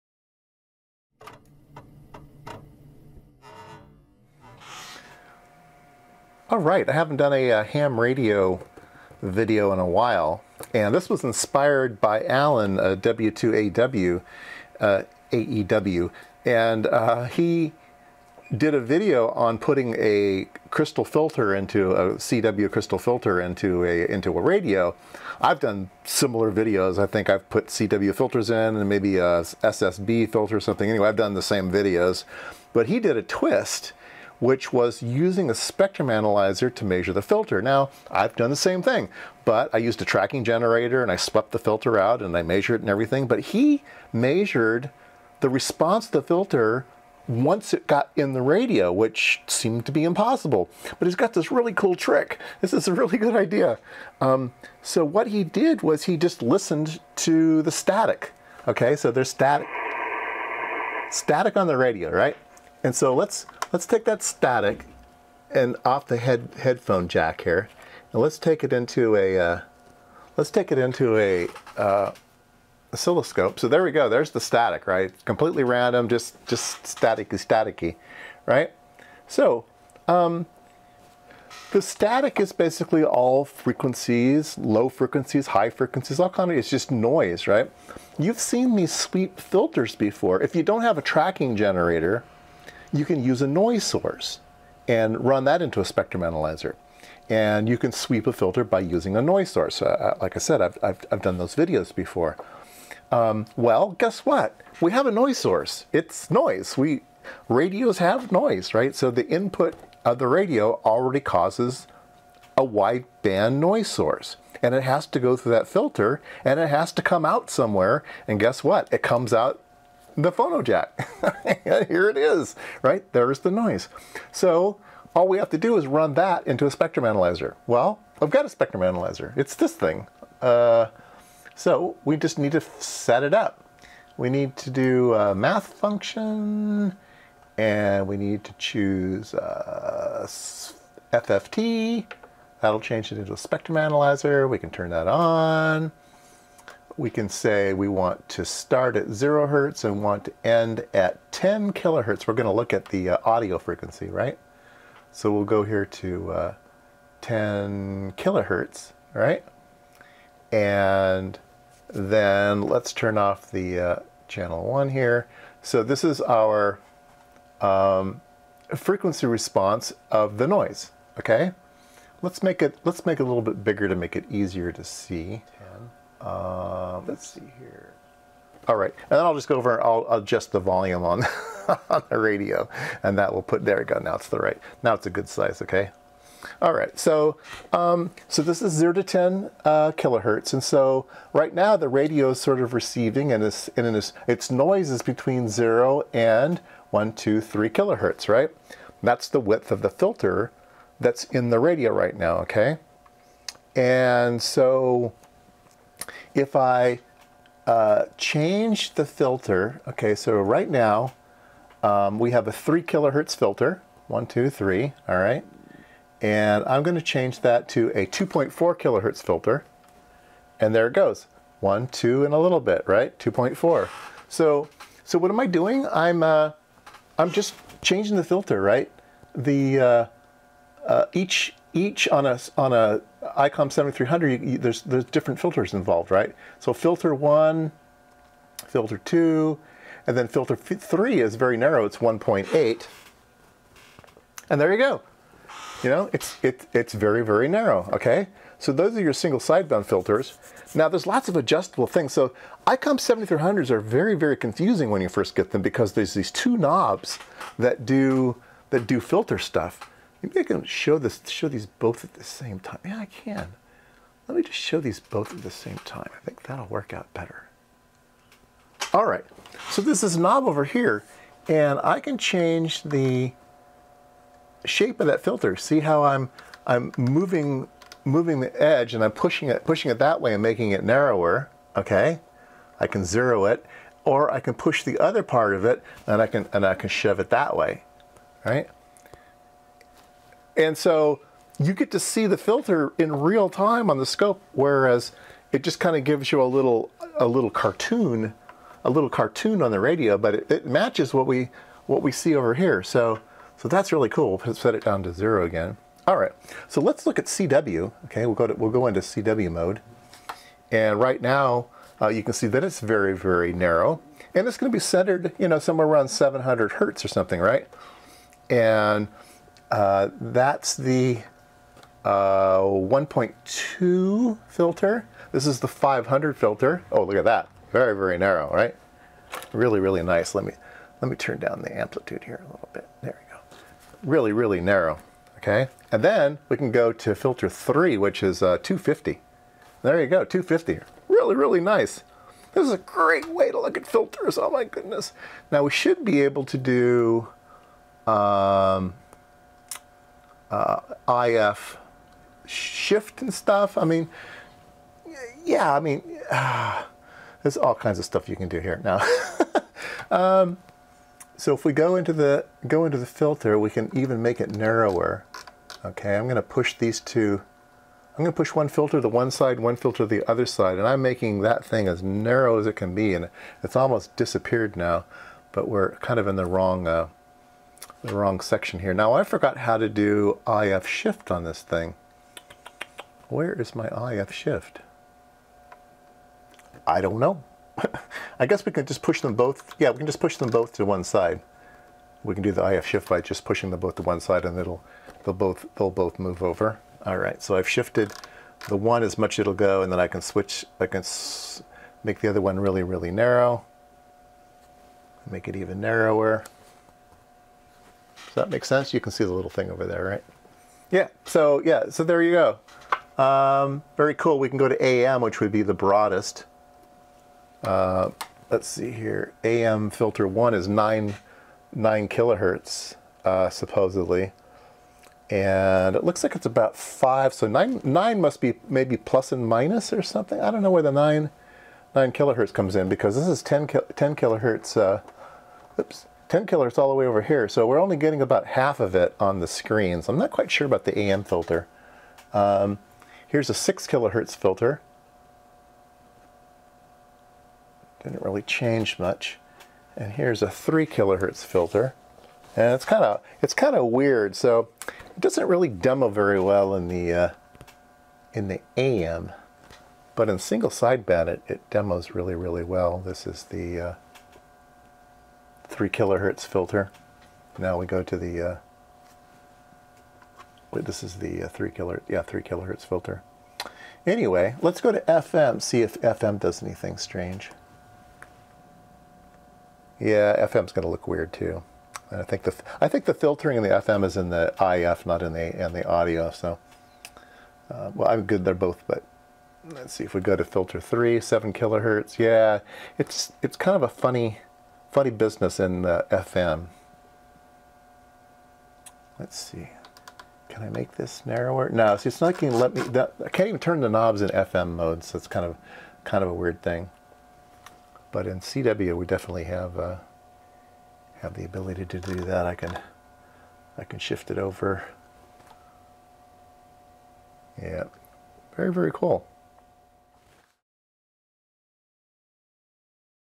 #840 ICOM 7300 Filters Viewed Real Time on a Spectrum Analyzer
Episode 840
We can look at all the amazing filters in the ICOM 7300 with my shiny new oscilloscope with a good FFT. Here is the older CW filter video by me